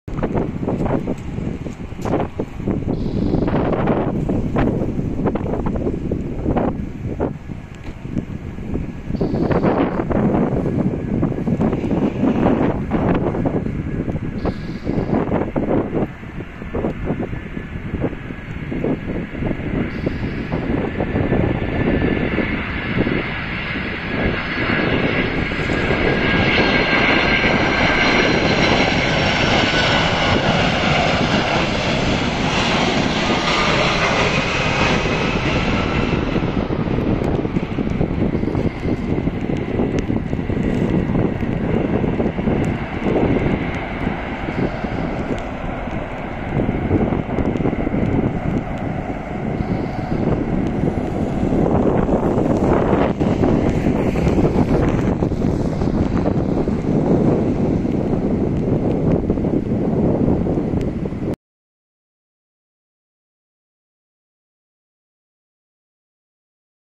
Rainy and Windy Approach | Lufthansa LH492 B744 | D-ABVX | FRA to YVR | 08L
Rainy and Windy Approach
Lufthansa LH492 B744
Boeing 747-430
Reg: D-ABVX
Frankfurt to Vancouver